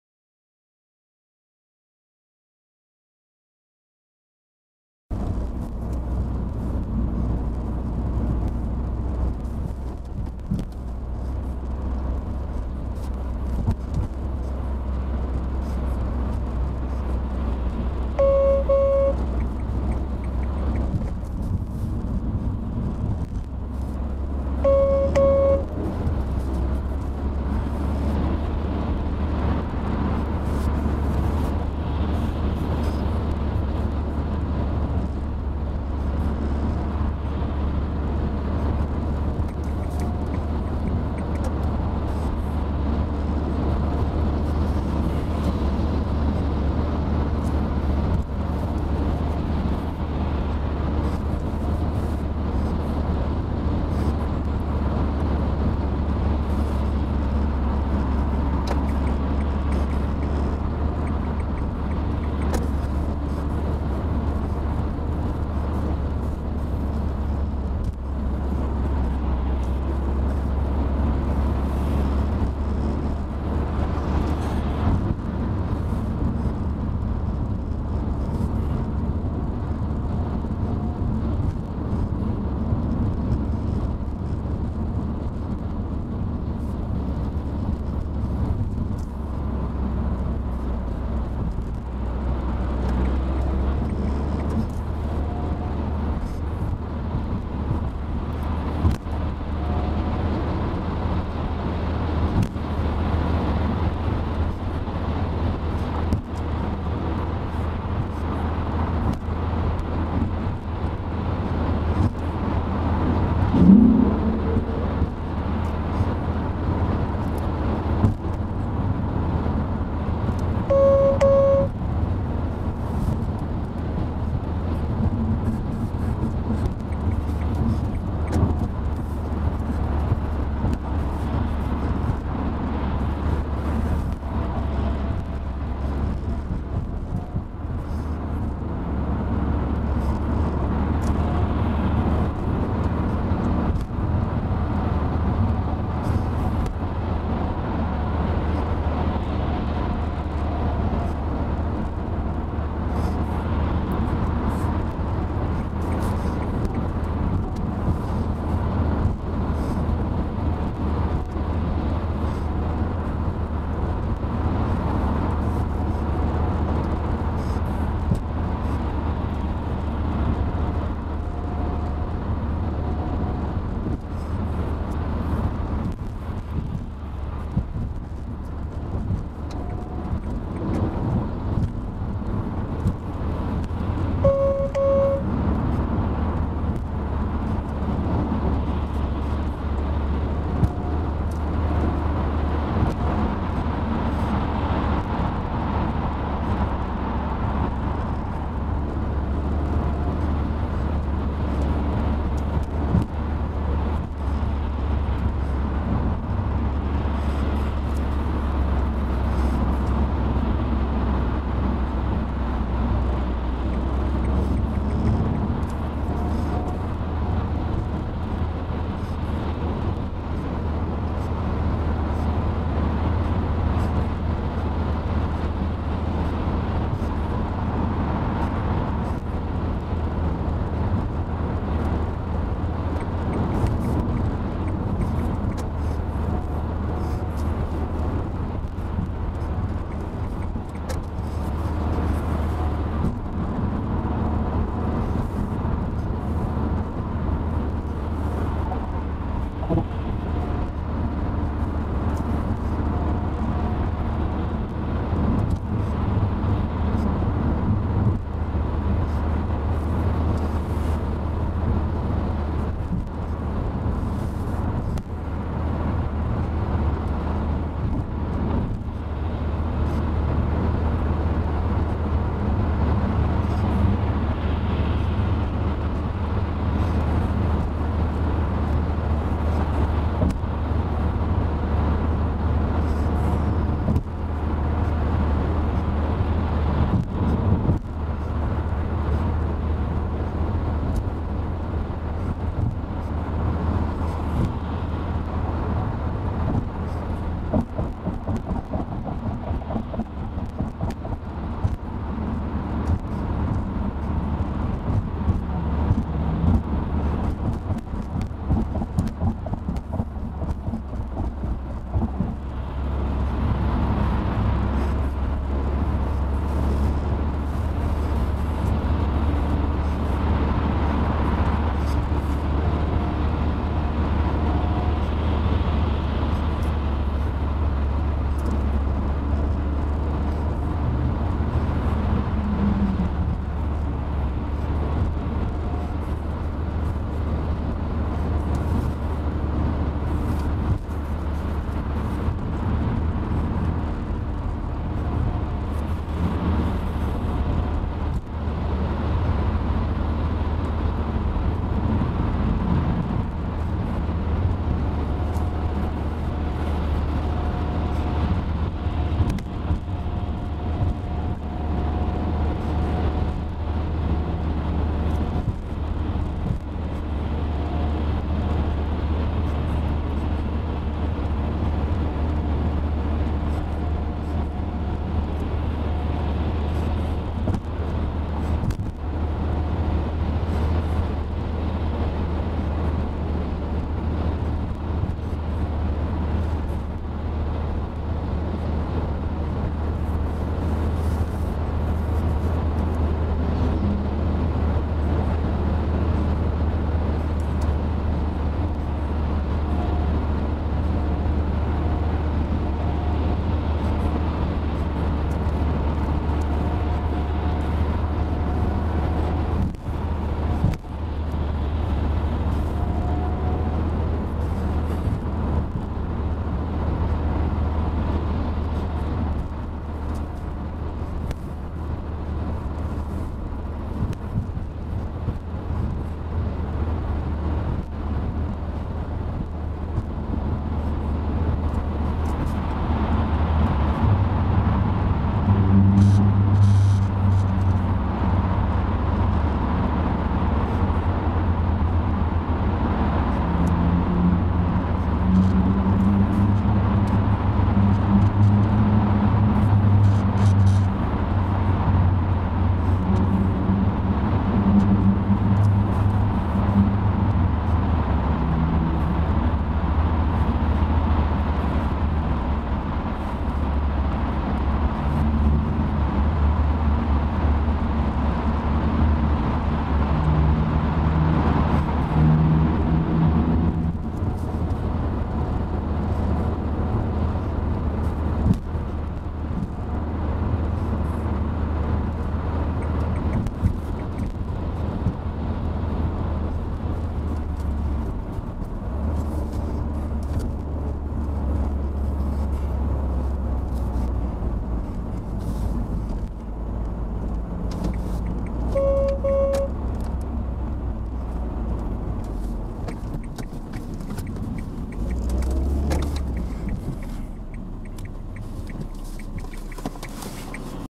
🇩🇪 A100: Seestraße - Grenzallee (2x)
Die A 100 in Berlin von (1) Seestraße bis (26) Grenzallee in Richtung Treptow.